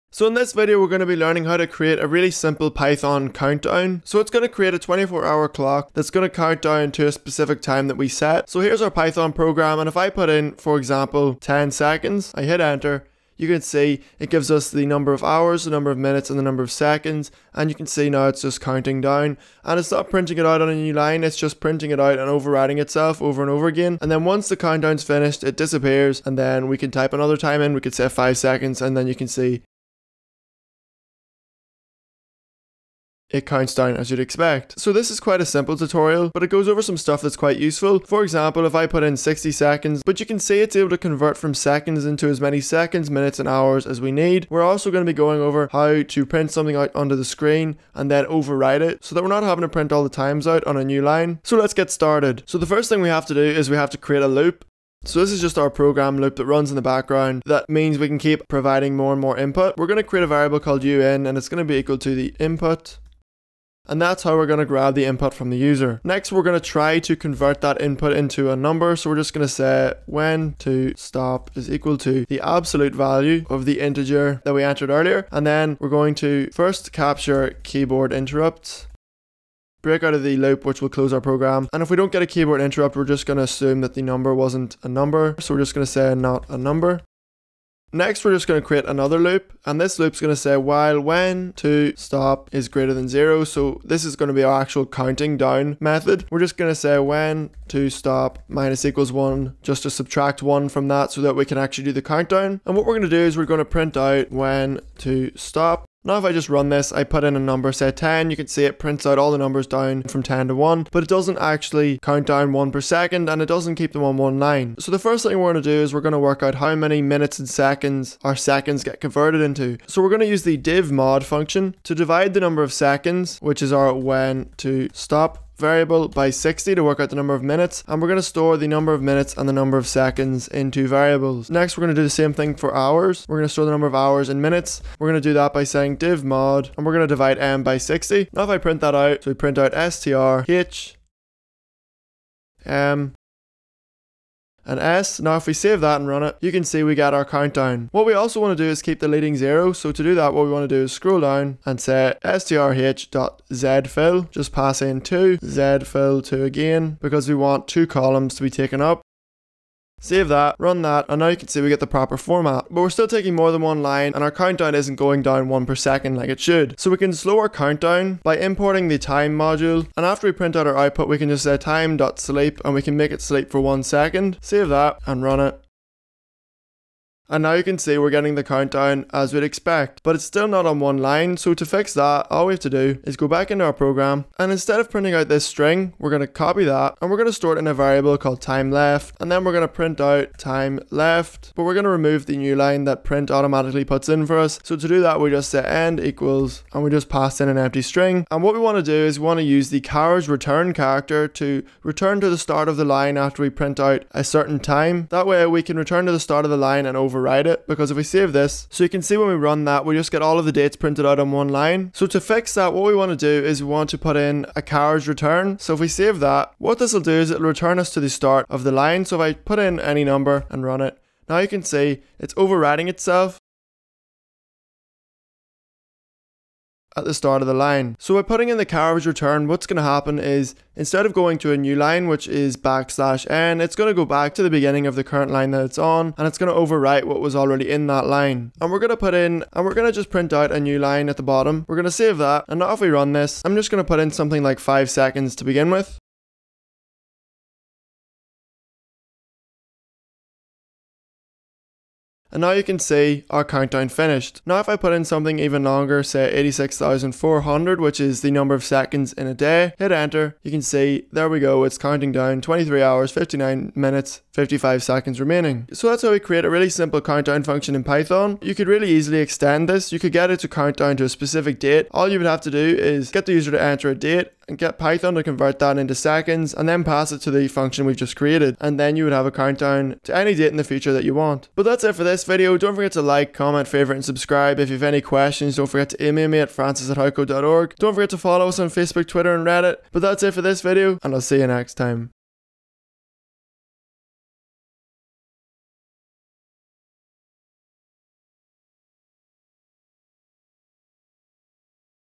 Simple Python Countdown
In this video we'll be learning how to create a simple Python countdown script.

Our countdown script allow us to enter any number of seconds to count down from, our script will then format the time remaining as a date.